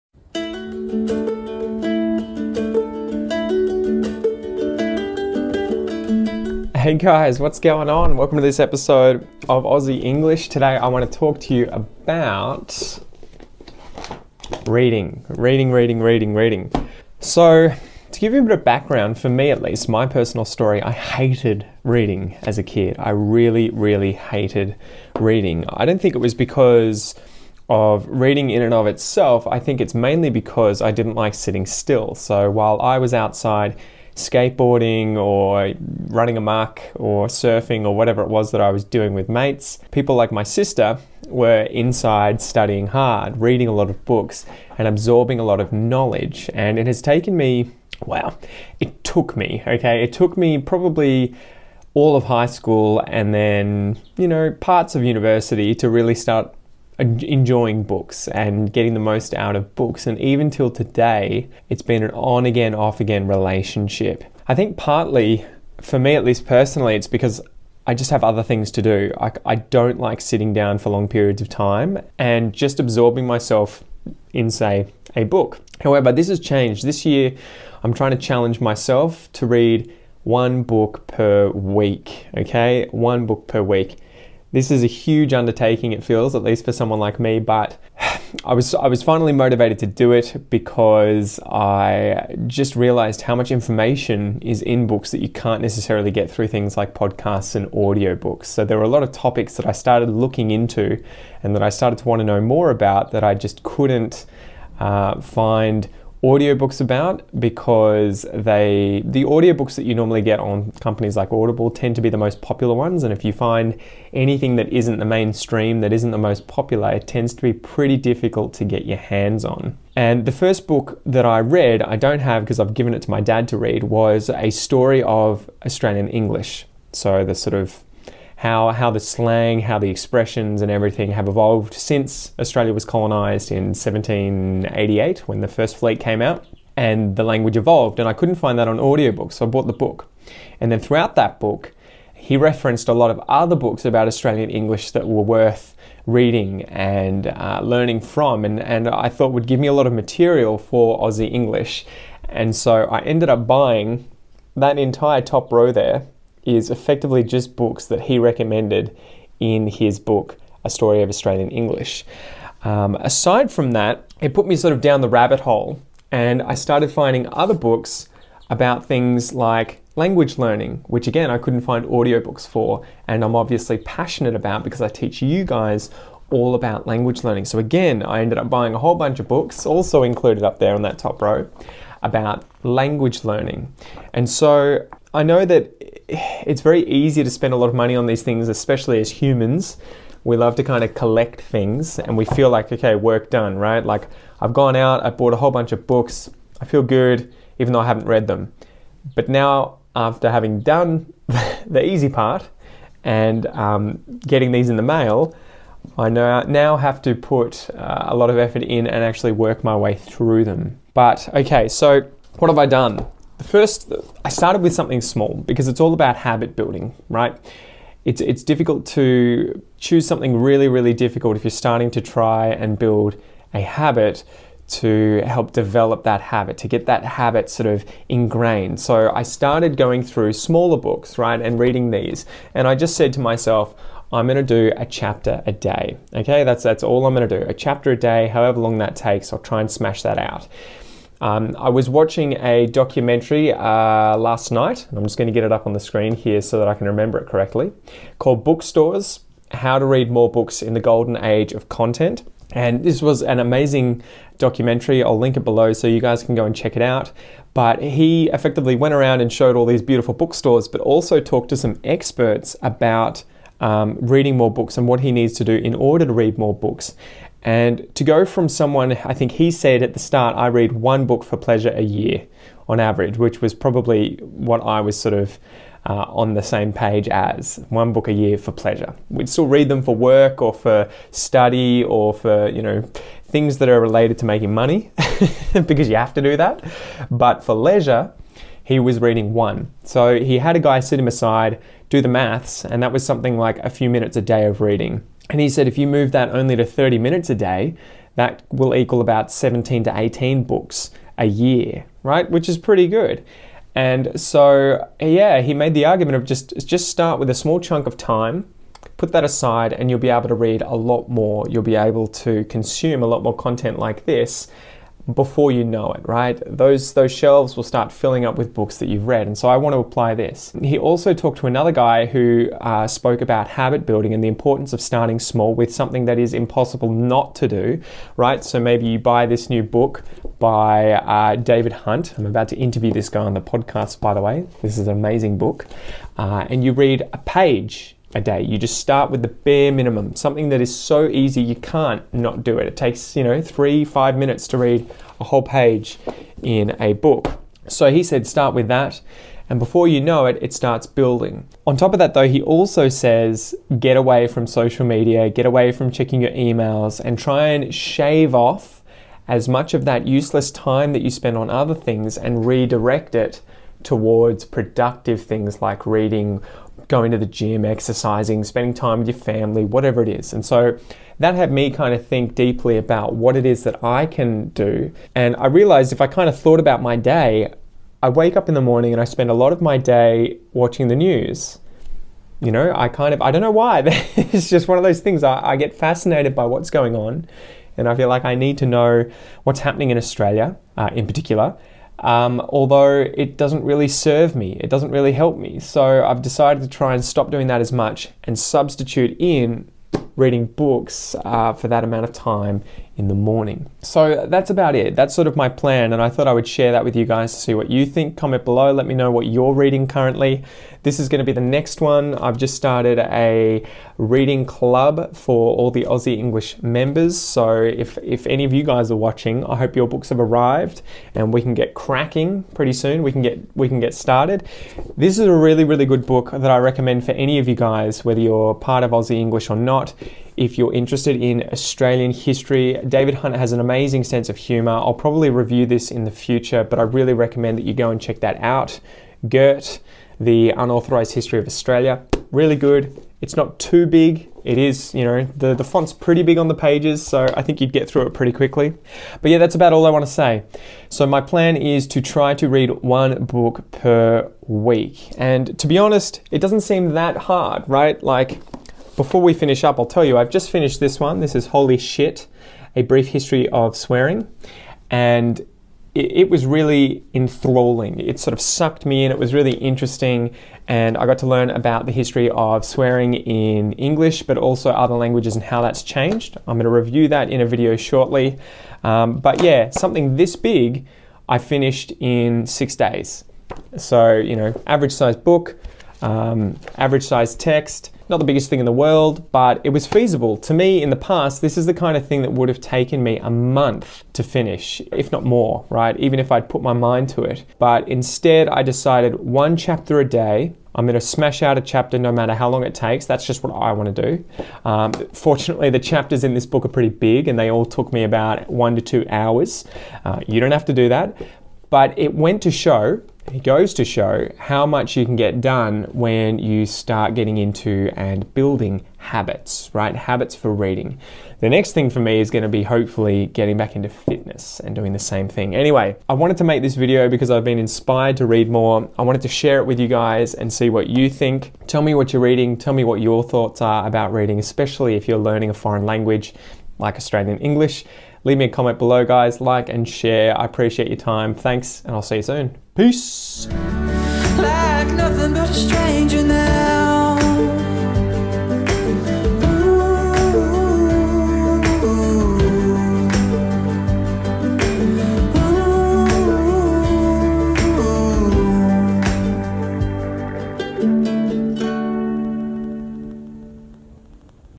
How to Read More Books... in English! | Tips for How to Read More!
In this episode of Aussie English I talk about how to read more books in English and give you some tips to help you start reading more English books!

If you're worried about how to read in English, how to read more, how to improving your English reading comprehension, check out this episode and I'll tell you about my plan for how I'm going to read one book per week for the rest of the year.